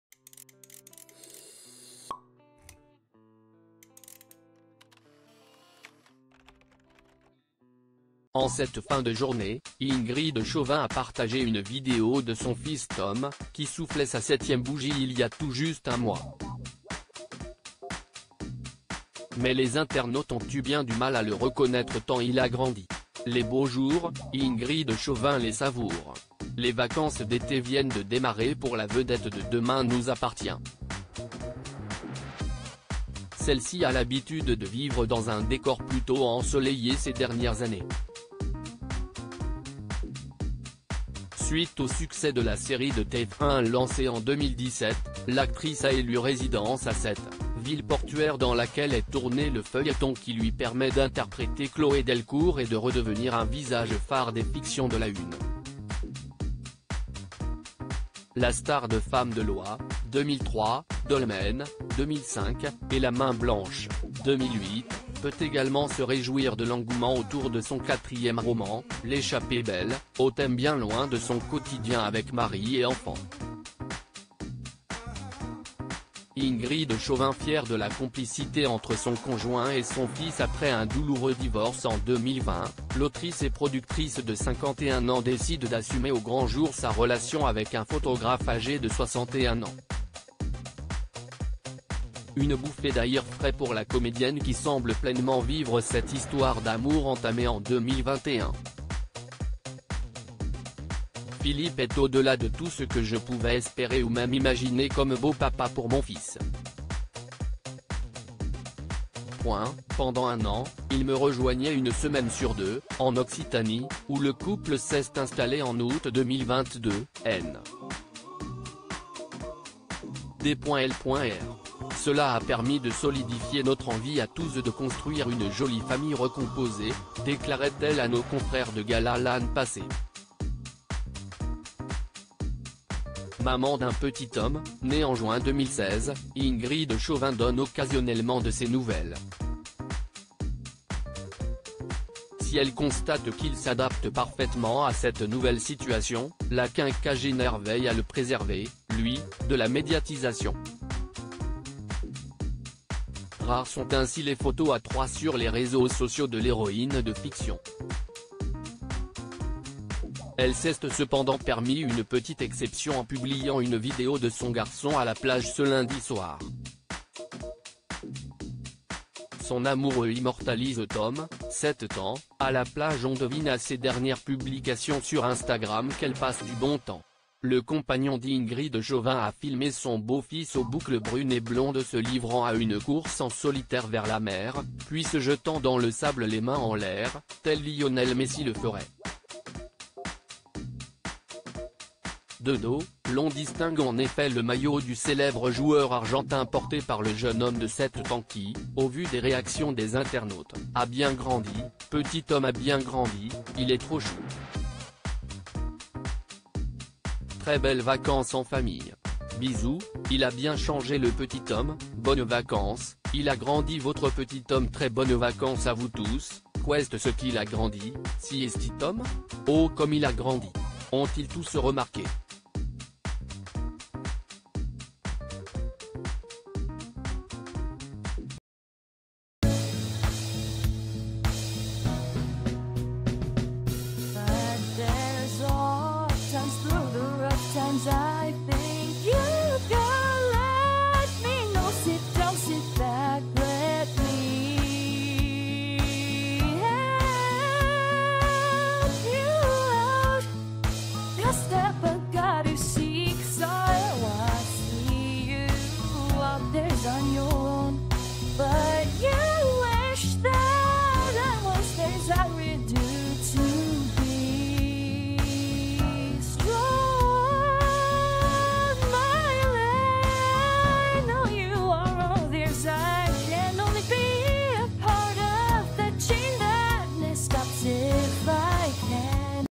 "C'est Tom ?" : Ingrid Chauvin partage une vidéo de son fils qui a bien grandi à la plage, les inter
"C'est Tom ?" : Ingrid Chauvin partage une vidéo de son fils qui a bien grandi à la plage, les internautes choqués par son évolution